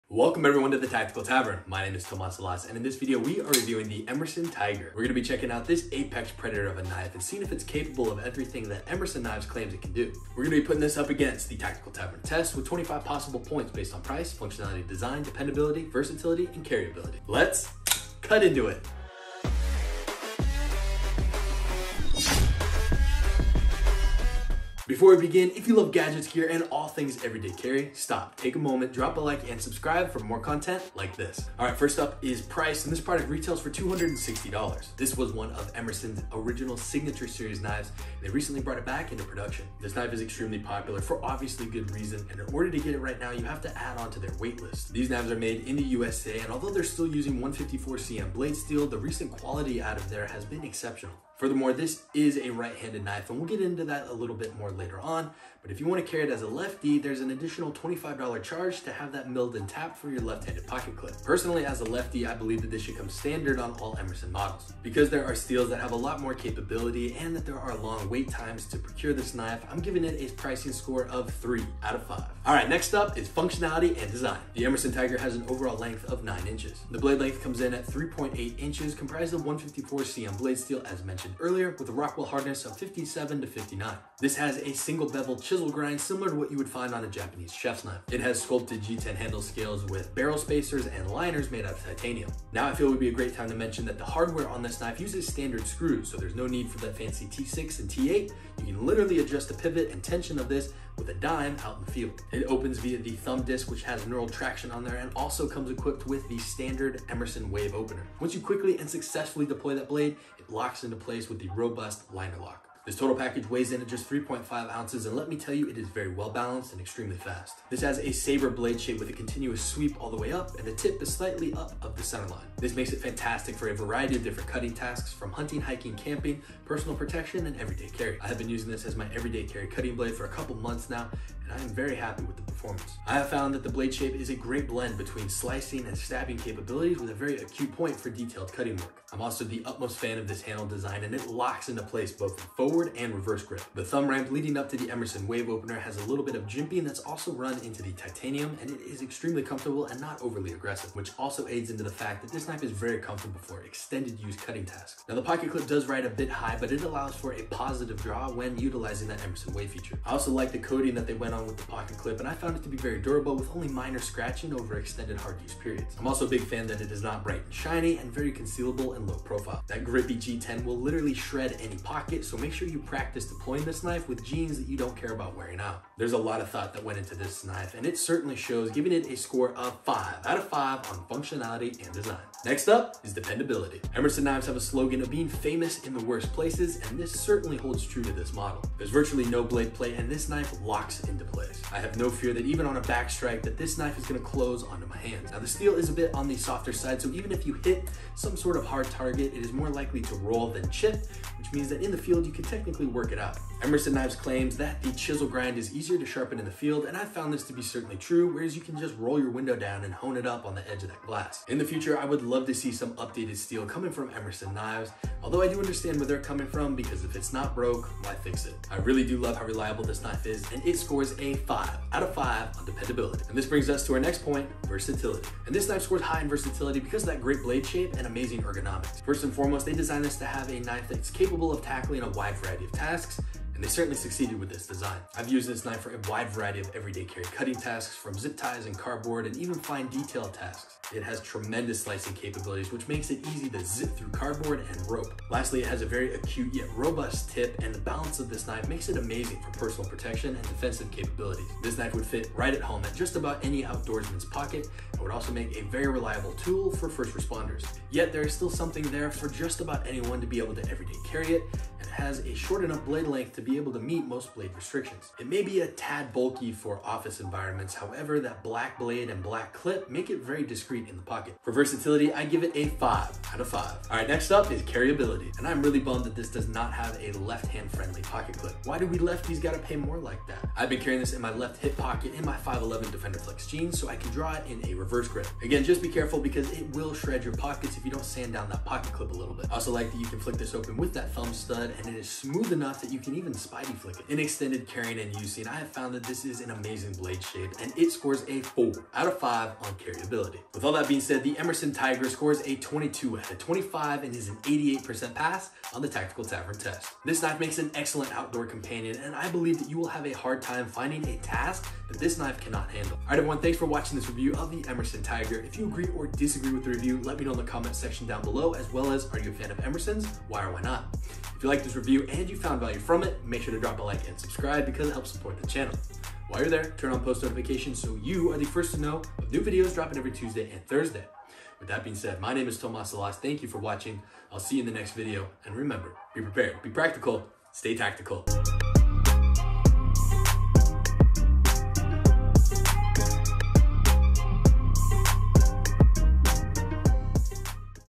Emerson Tiger King Of The Urban Jungle? Emerson Knives Review |An Ernest Emerson Favorite!
With the release of Tiger King Season 2 coming out on Netflix I was inspired to share my thoughts on this Emerson Knife!

The Emerson Tiger claims to be the Apex predator in the tactical knife world. Said to be capable of taking down a wide range of everyday carry cutting tasks, but is it actually worth the money or all for show?

In this video we are reviewing this classic Emerson Knives Inc. blade who brought you famous models such as the the CQC-7, CQC-13, Combat Karambit, Elvia and CQC-8 (Close Quarter Combat). The long sweeping blade on this Emerson allows for deep cuts and a fine point for detailed cutting work such as popping open zip ties.

Let’s find out if its all looks or if that roar is backed by bite making it a true Tiger King!

Be Prepared, Be Practical, & Stay Tactical!